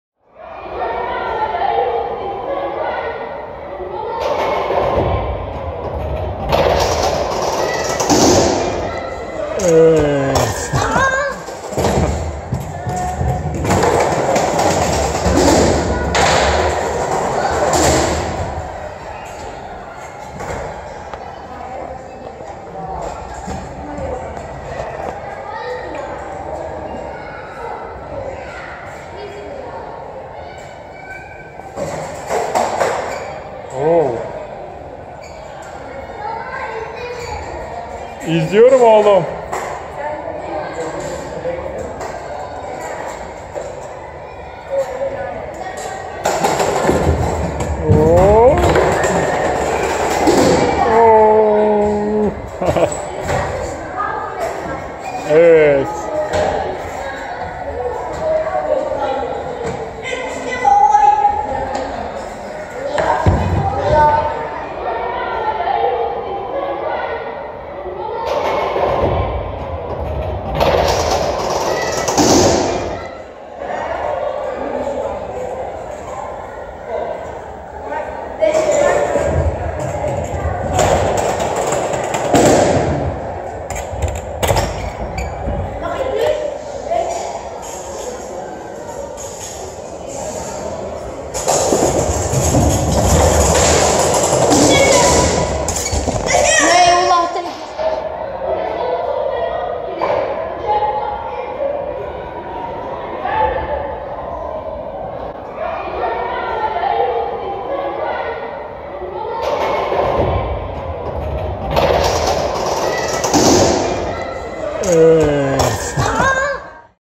Amsterdam'da Kapalı Oyun Alanı, Kaykay Keyfi
Amsterdam ziyaretimizde oyun alanına dönüştürülmüş eski bir metro istasyonuna gittik. Buradaki kaykay pistinde birkaç kayma denemesi yaptı Kuzey Alp. Çocuk videoları kanalımızda Kuzey Alp'in kaykay denemesini sizlerle paylaştık. İyi seyirler.